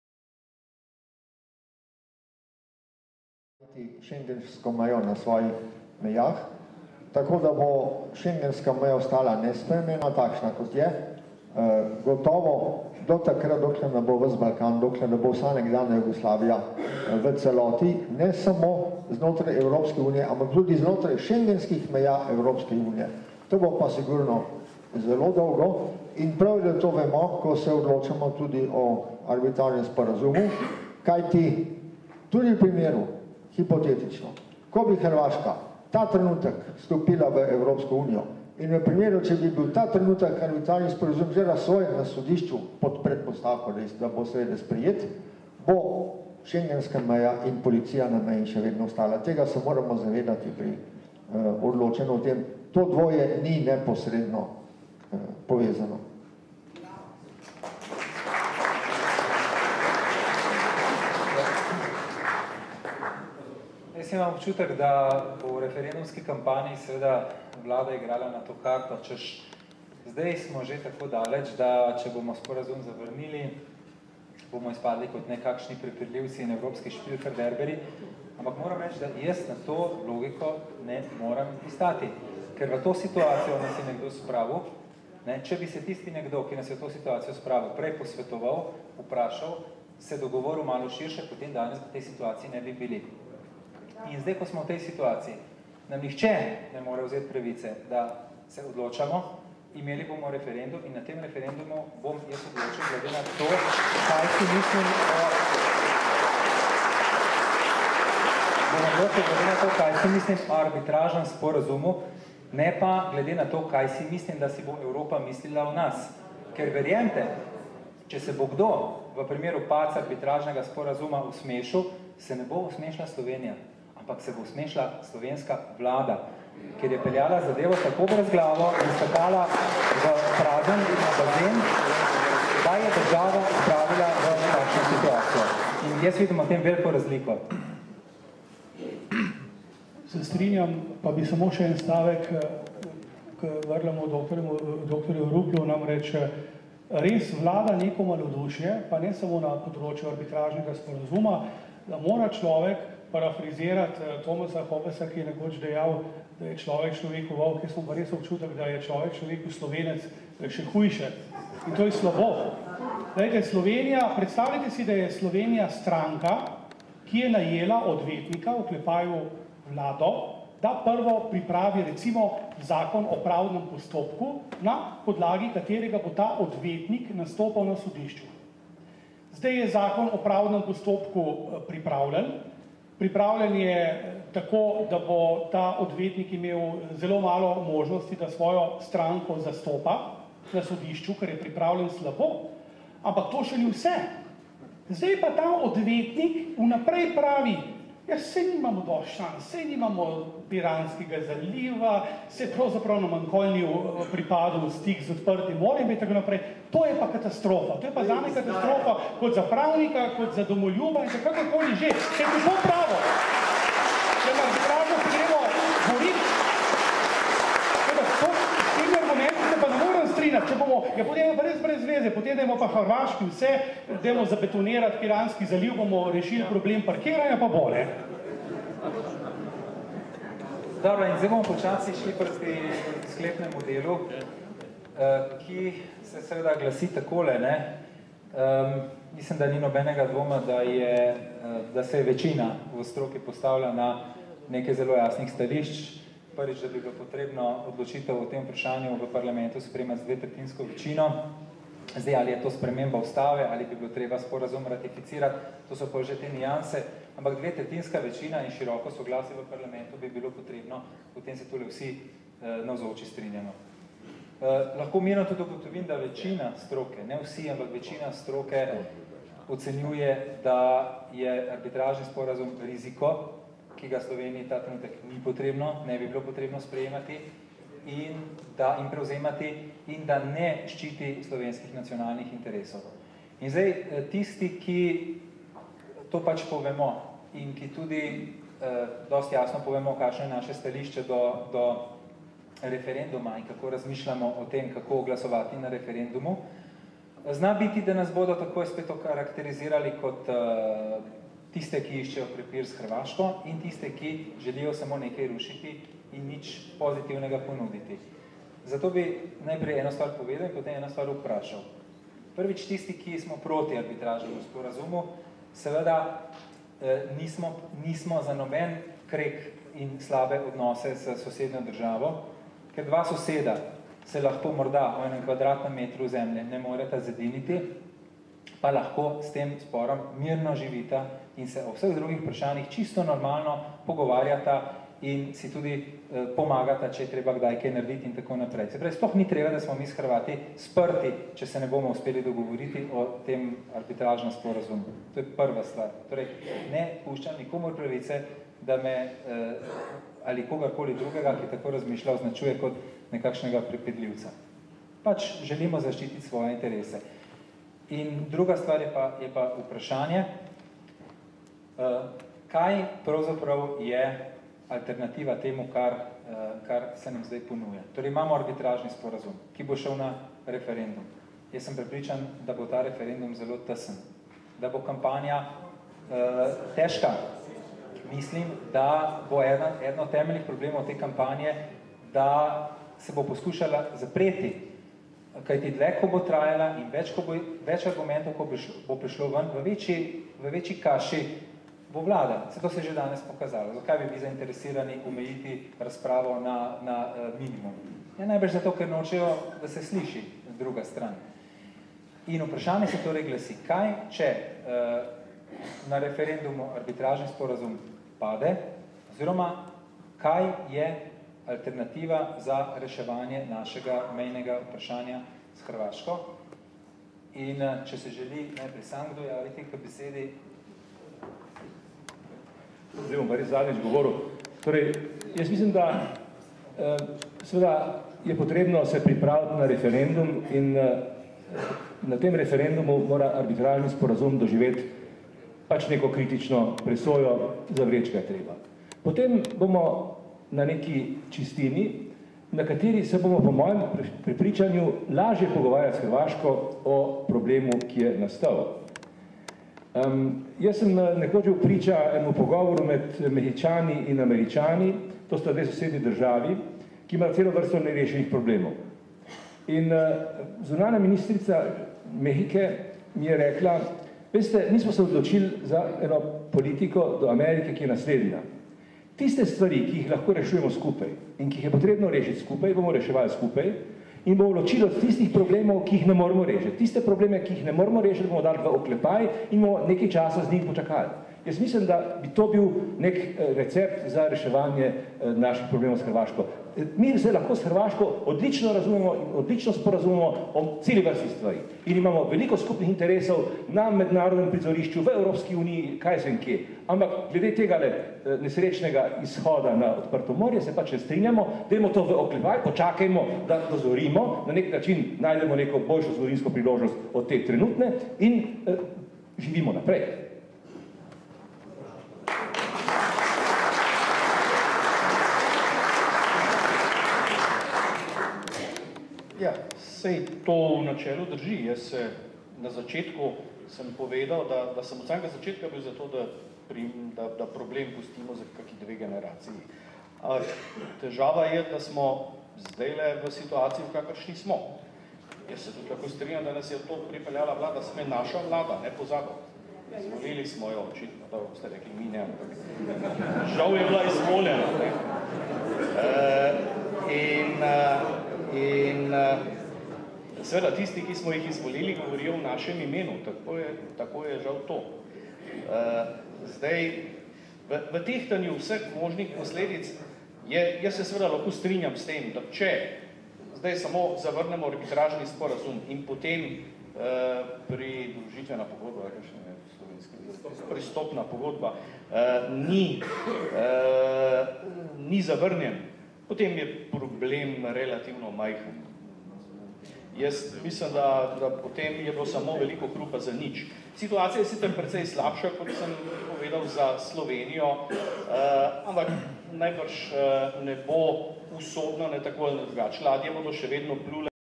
Zbor za republiko - okrogla miza: Arbitražni sporazum - rešitev ali napaka? (11.del)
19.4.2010, Ljubljana; Zbor za republiko je organiziral okroglo mizo z naslovom "Arbitražni sporazum - rešitev ali napaka?". Dogodek je povezoval in vodil predsednik Zbora za republiko, Dr. Gregor Virant. Kot razpravljavci so sodelovali še: Dr. Dimitrij Rupel, Dr. Lovro Šturm, Dr. Rajko Pirnat, Dr. Marko Pavliha, Dr. Miro Cerar in Dr. Tone Jerovšek.